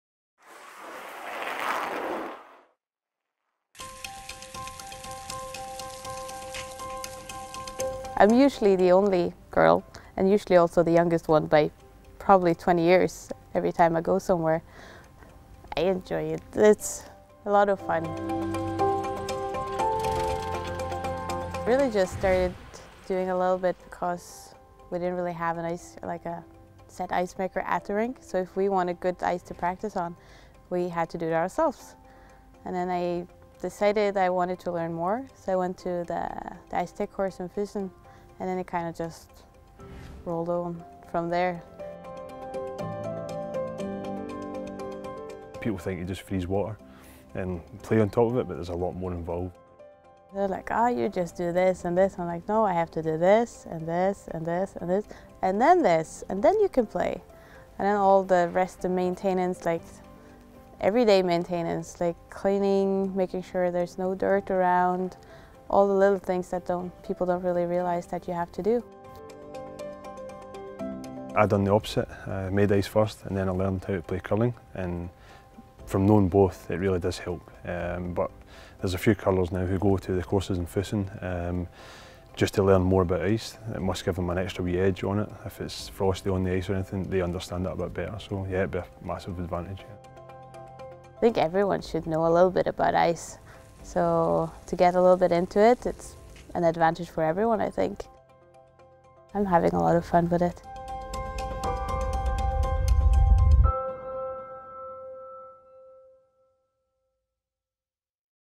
Around the House: Ice maker, Ingvild Skaga
Norway's Ingvild Skaga is one of World Curling's only female ice makers. As an athlete, when she didn't have adequate ice conditions to practice on there was only one solution - do it yourself.

World Curling TV met Ingvild in Helsingborg, Sweden, to find out more about her journey into ice making.